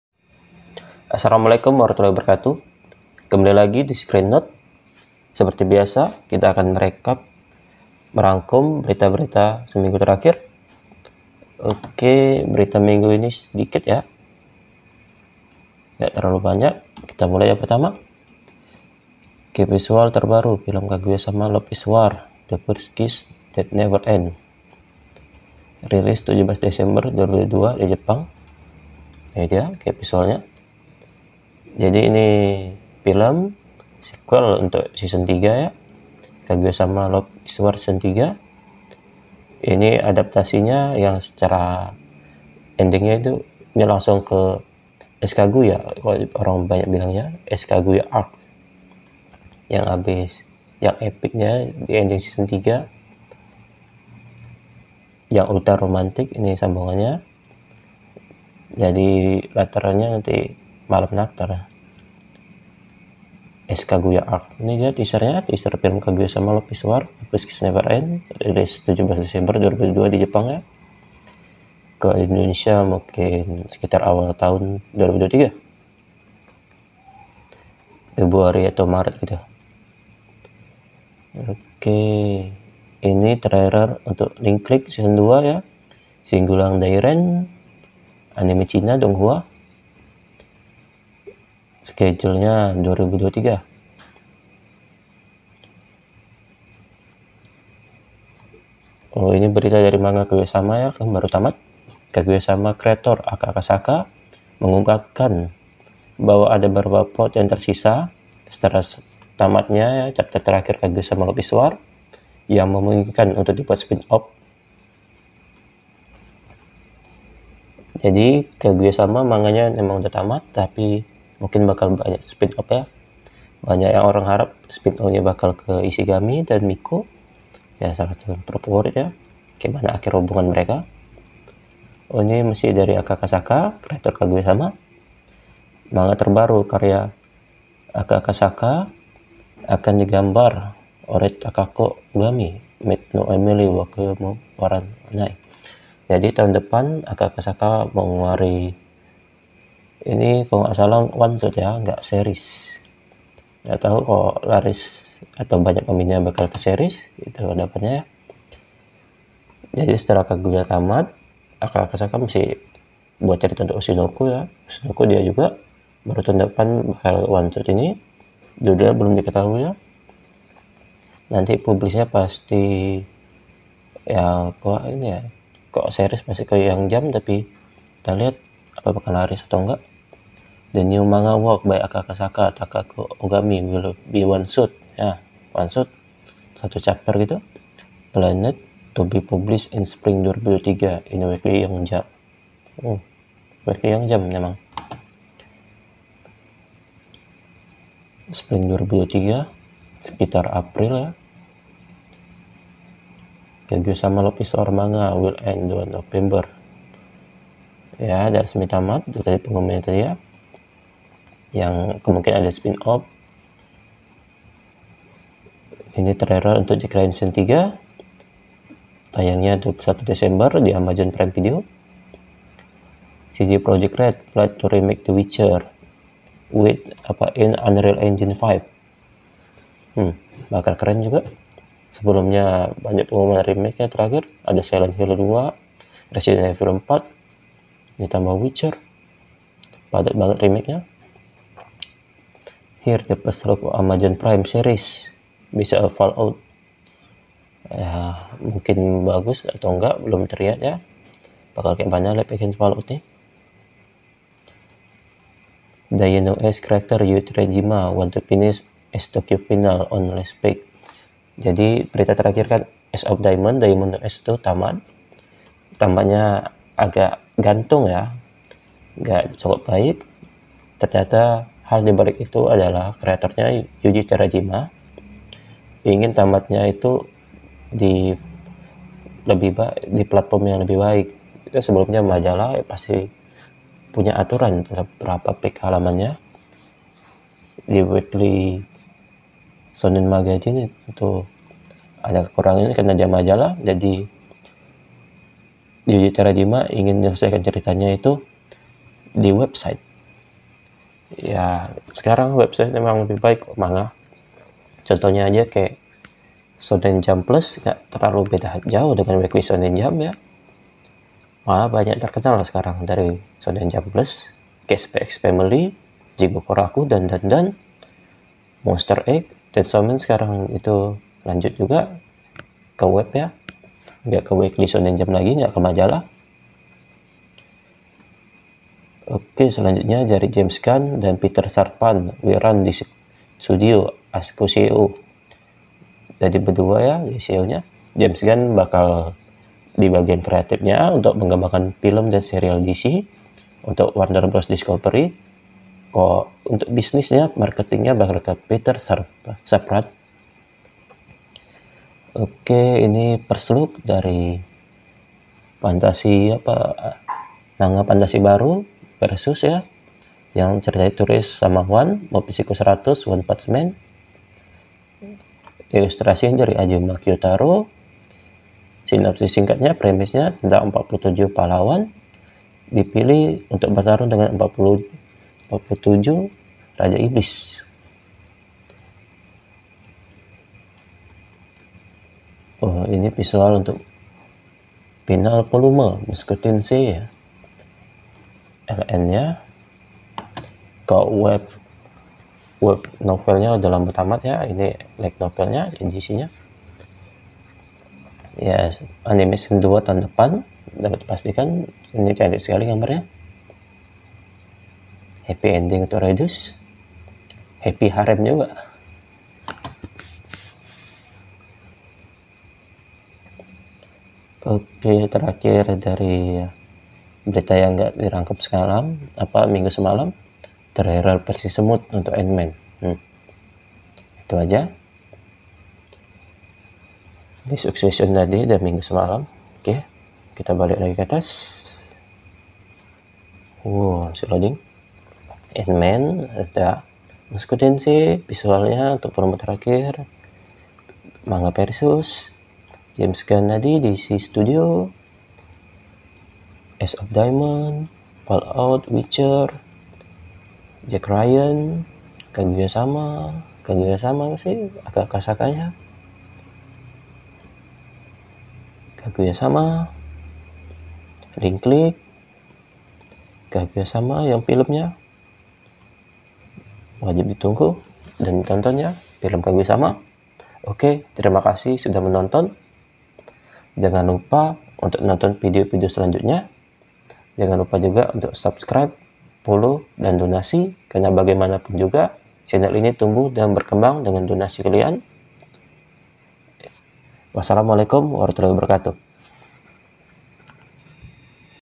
[BERITA] KEY VISUAL TERBARU DAN INFO TANGGAL RILIS FILM KAGUYA-SAMA: LOVE IS WAR
Berita terbaru dari dunia anime:
- Key Visual terbaru film Kaguya-sama: Love Is War -The First Kiss That Never Ends- (info tanggal rilis)
- Trailer Link Click S2 (info jadwal tayang)
- Info manga Kaguya-sama: Love is War (potensi spin-off cerita)
- Info manga terbaru Aka Akasaka (author Kaguya-sama: Love is War, Oshi no Ko)
- Info terbaru tentang lanjutan manga Ace of Diamond
- Dan berbagai berita lainnya seminggu terakhir

CREDIT:
- Diproduseri oleh: Sultan Yardi Hijrah (SYH)
- Disuarakan oleh: Sultan Yardi Hijrah (SYH)
- Gambar Outro: Solo Leveling Manhwa